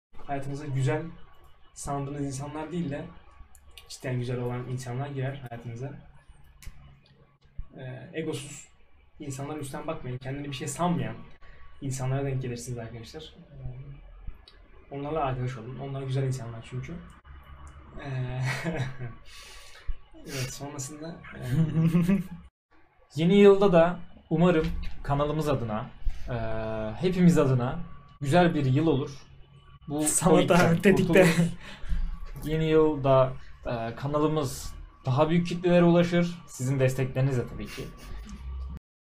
Eren abi Ve Cihan abi Yeni Yıldan beklentilerini açıklıyor - Hayatımıza Girecek İnsanlar ve Kanal
Bu yayında siz izleyicilerimizden videolar istemiştik , gönderen izleyicilerimize çok teşekkür ediyoruz ve sizlere yeni yıldan beklentilerimizi sunuyoruz.